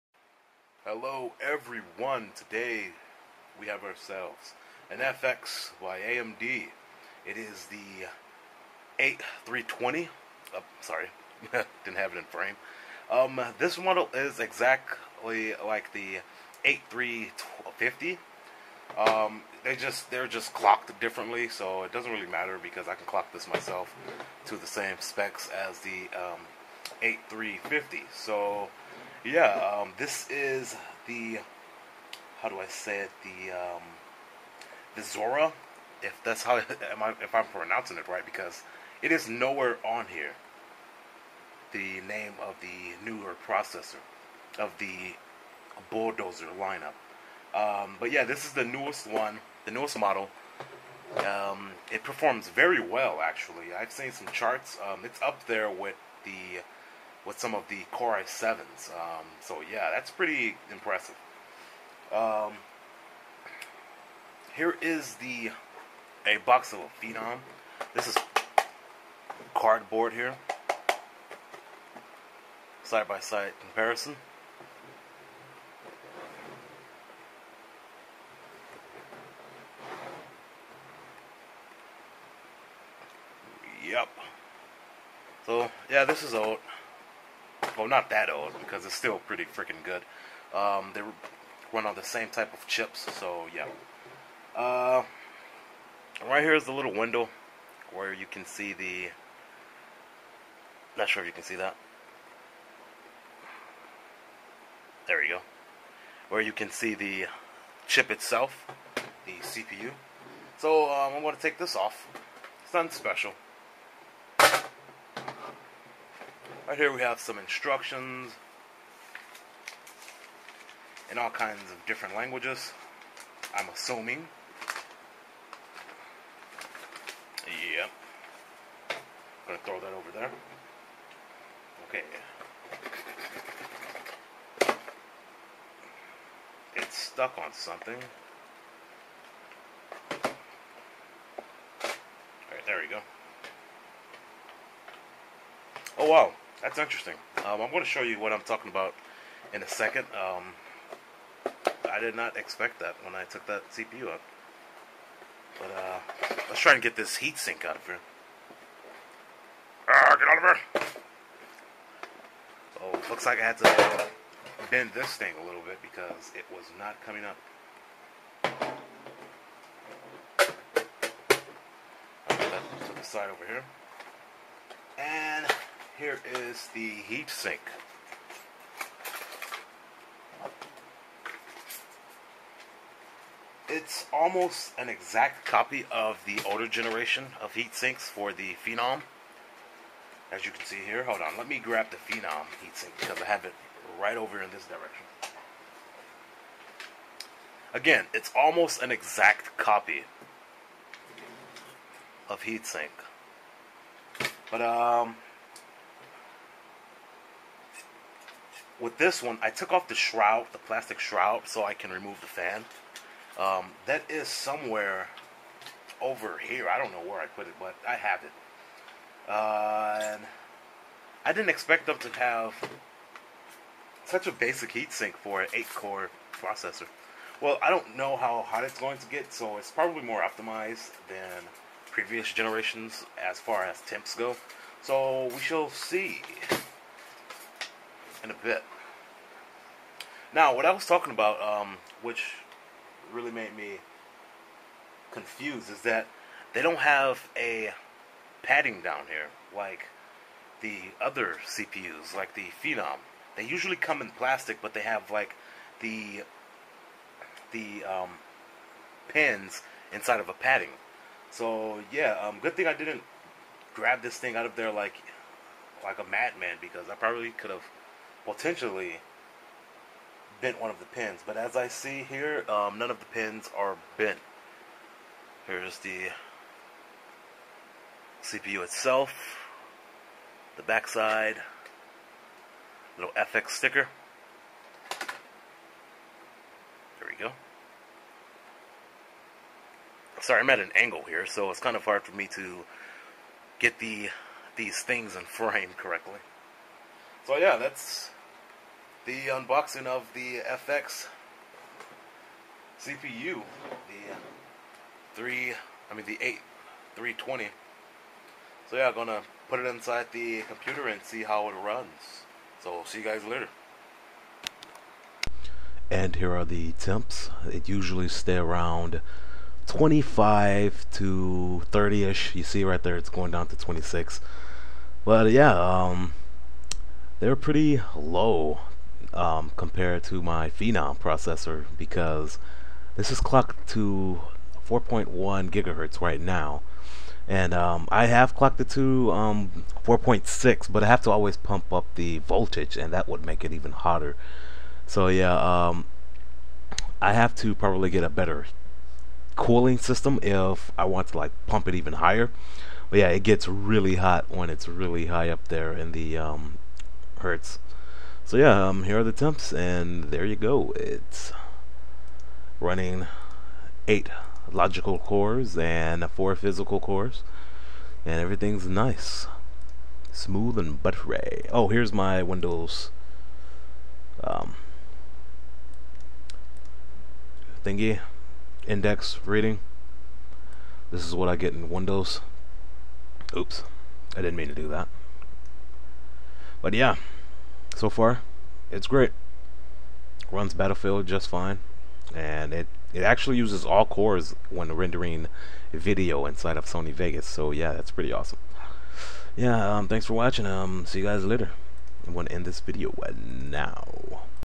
Unboxing and Review- AMD FX-8320 Vishera (also check out the temps)
I have to say the new Vishera gets crazy temps compared to the previous Phenom chips. My FX-8320 at complete idle with nothing running I can pull temps as low as 16c while clocked at 4.2, yes you heard me 16c. Don't believe me here is a link  .

But normally when I am actually doing stuff on the computer like in this video it will bump up to 25c or 30ish with a load temp of 50c. I'm loving this cpu so far, it uses all cores in Sony Vegas which is nice.

Specs:

AMD FX-8320 Vishera 4.6GHz Coolit ECO Liquid cooler

patriot 12GB 2000mhz DDR3 (clocked @1800mhz)

EVGA GTX 660 ti GDDR5 2GB

x2 ASUS VE258Q monitors

GIGABYTE GA-990FXA-UD3 AM3+AMD 990FX SATA 6Gb/s USB 3.0 ATX AMD Motherboard

OCZ Vertex 4 128GB SSD plus Western Digital Caviar Blue Hard Drive 1TB, 7200RPM

950watt psu

NZXT Phantom PHAN-001WT White Steel case

Microsoft Sidewinder x5 Gaming Mouse

Corsair Vengeance 1500 Headset